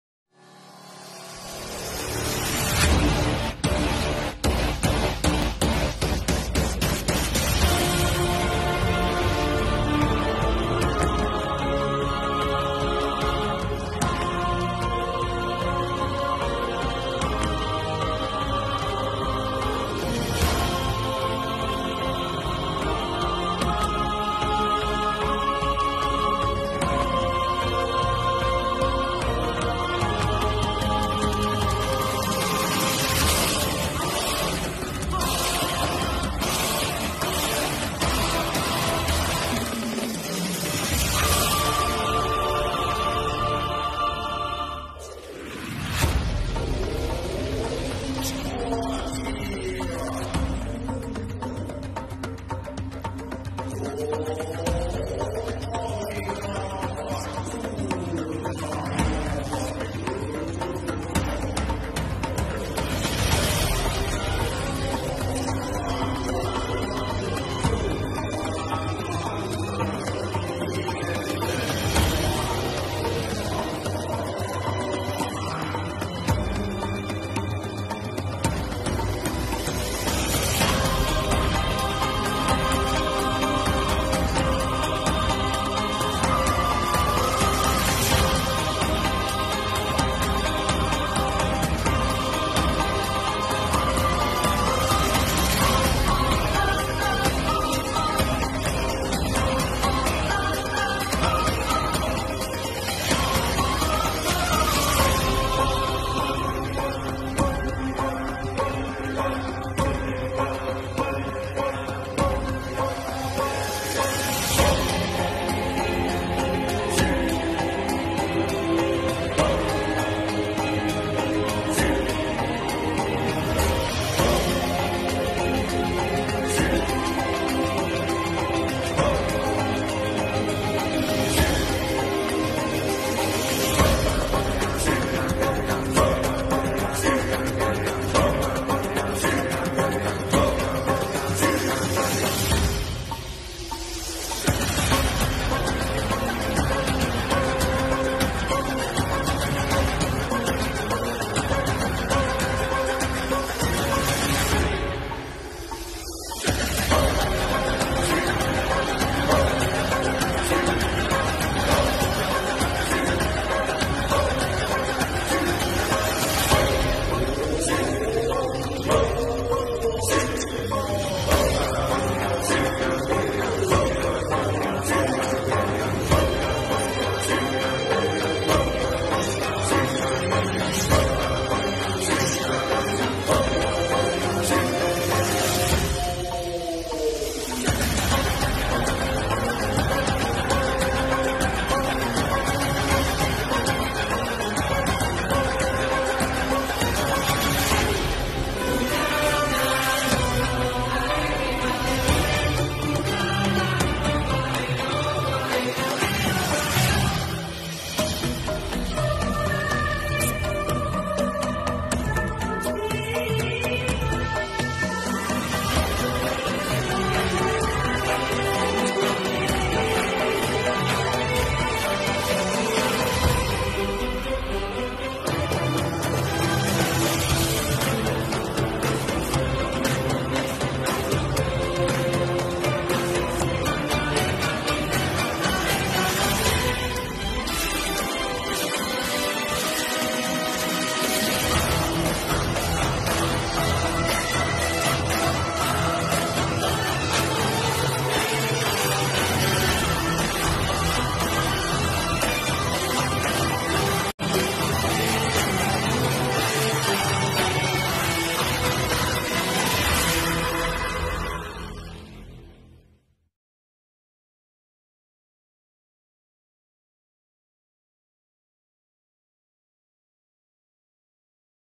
Pergelaran Budaya Nusantara Pada Gala Dinner di GWK Bali - Indonesia
Pergelaran Budaya  Nusantara Pada Gala Dinner di GWK Bali - Indonesia  dan Jamuan Makan Malam oleh  Presiden Jokowi Dodo kepada Delegasi Negara KTT G20 .